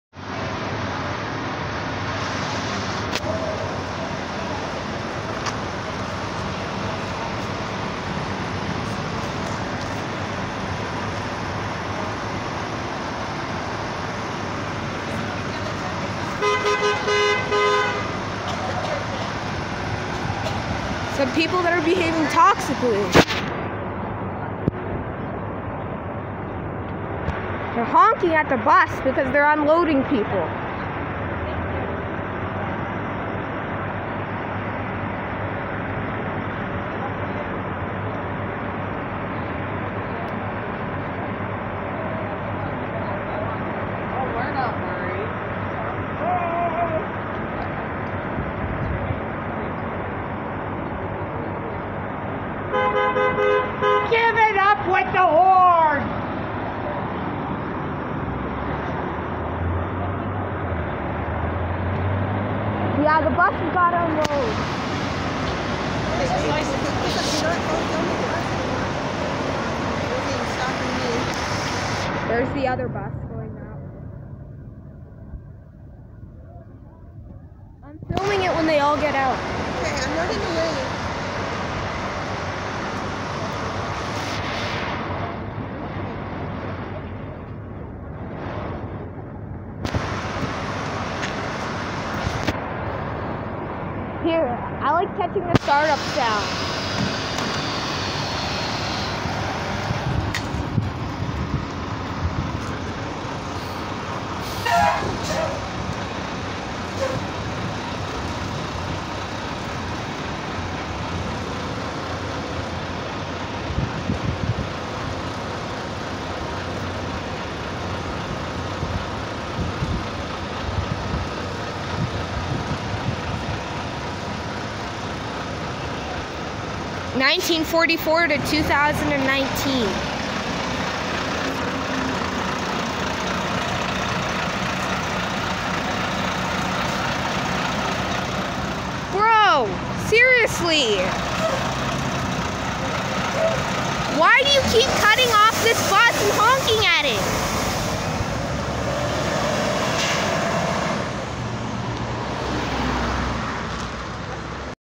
Idiot honks at Hammond bus unloading passengers near the Midland butter tart festival
A lot of buses come during the butter tart festival